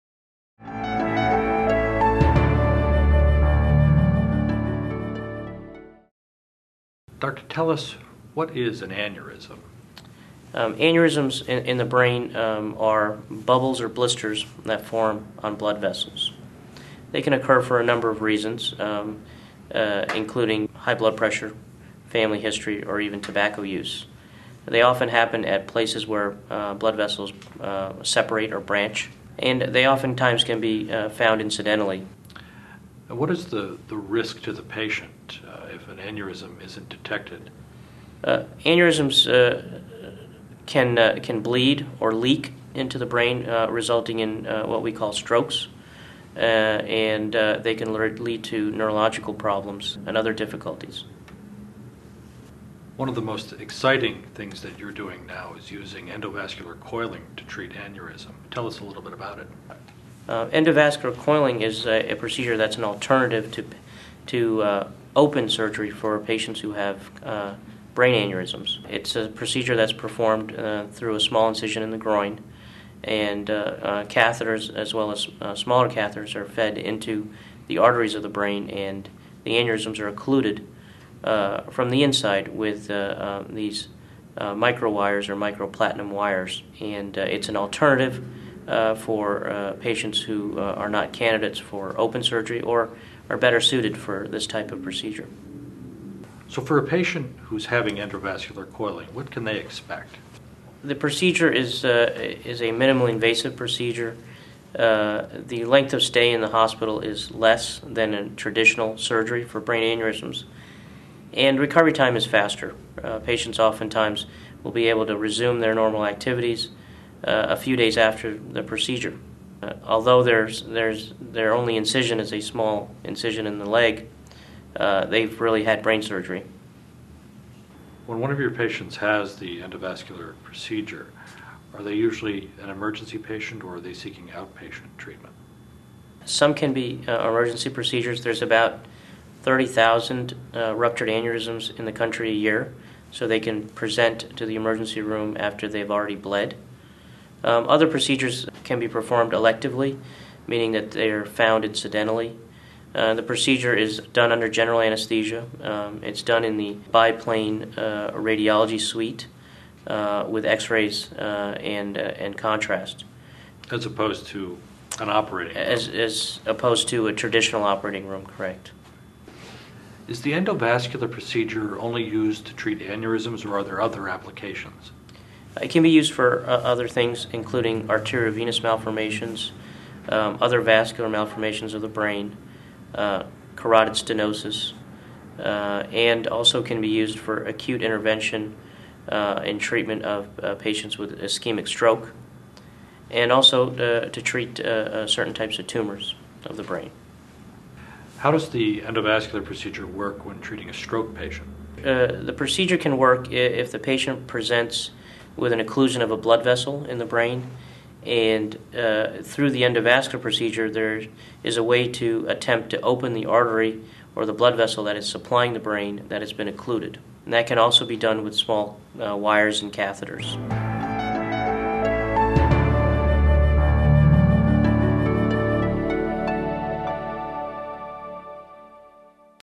Medical Moment: Neurosurgery - Aneurysms and endovascular coiling
Neurosurgeon Akash Agarwal, M.D., discusses brain aneurysms and one of the latest treatment options -- endovascular coiling.